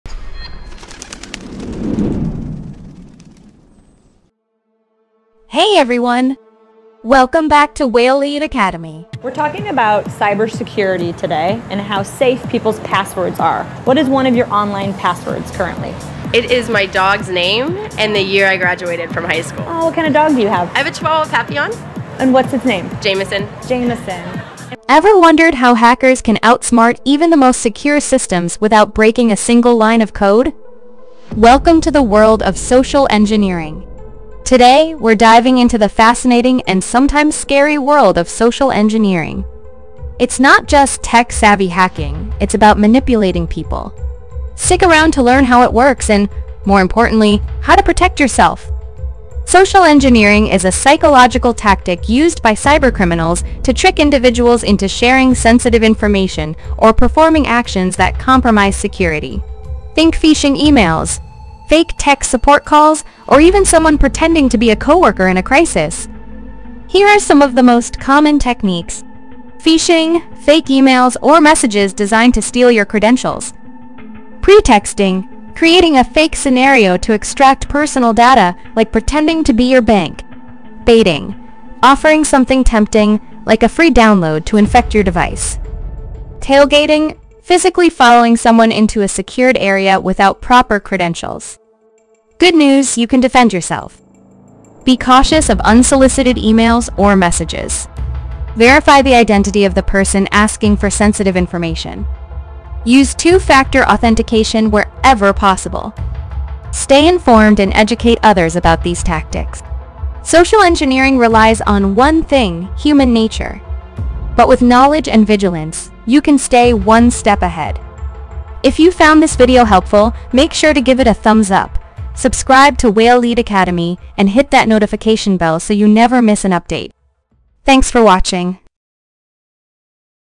Social Engineers EXPOSED The TOP SECRET Tactics They Use
In this video, we dive deep into the world of social engineering — the psychological manipulation of people into revealing confidential information or performing actions that compromise security.

🧠 Learn how social engineers exploit human behavior, trust, fear, and urgency to bypass even the most secure systems — without ever touching a line of code.

🔐 Topics covered:

    What is social engineering?

    Common techniques (phishing, pretexting, baiting, tailgating, etc.)

    Real-world examples of successful social engineering attacks

    How to recognize and protect yourself from manipulation

    Insights into the mindset of a social engineer

This is a must-watch for anyone interested in cybersecurity, ethical hacking, or digital awareness.

💬 Got questions or ideas? Drop them in the comments — we reply to everyone!